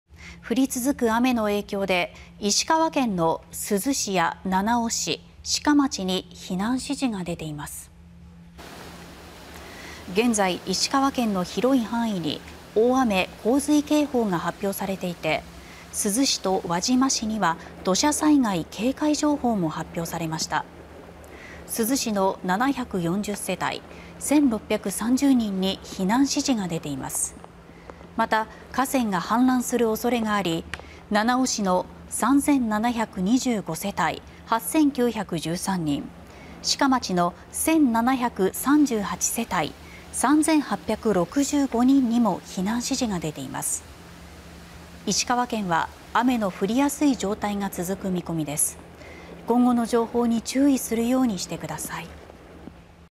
【速報】石川・七尾市などに「避難指示」 河川氾濫の恐れ(2022年9月1日)
降り続く雨の影響で石川県の珠洲市や七尾市、志賀町に避難指示が出ています。

　現在、石川県の広い範囲に大雨・洪水警報が発表されていて、珠洲市と輪島市には土砂災害警戒情報も発表されました。

　珠洲市の740世帯、1630人に避難指示が出ています。また河川が氾濫する恐れがあり七尾市の3725世帯8913人、志賀町の1738世帯3865人にも避難指示が出ています。

　石川県は雨の降りやすい状態が続く見込みです。今後の情報に注意するようにして下さい。